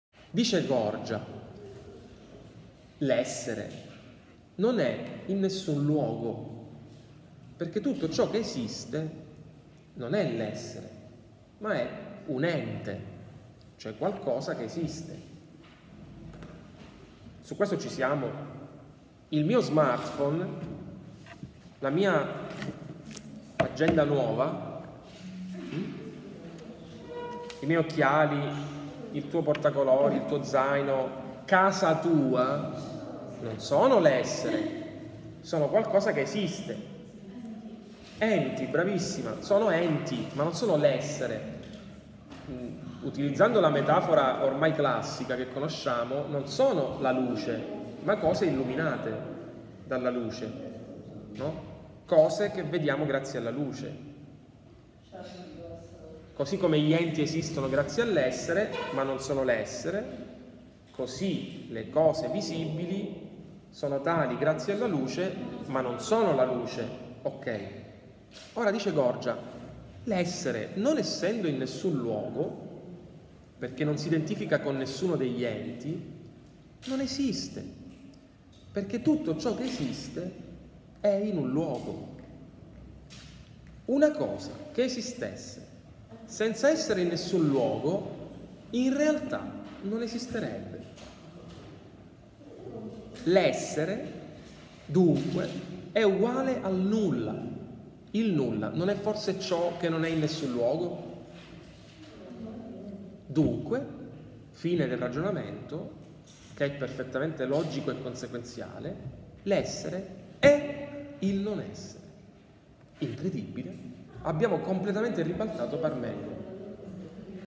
GORGIA: L’ESSERE NON ESISTE
Il grande Parmenide, seguendo un rigoroso ragionamento, aveva concluso che l’essere è e il non essere non è. Il sofista Gorgia, adesso, con un ragionamento altrettanto coerente, arriva a ribaltare totalmente Parmenide, giungendo alla sconcertante conclusione che l’essere non esiste e che, dunque, coincide con il nulla. È il cosiddetto nichilismo, che vediamo qui nascere per la prima volta nel pensiero occidentale.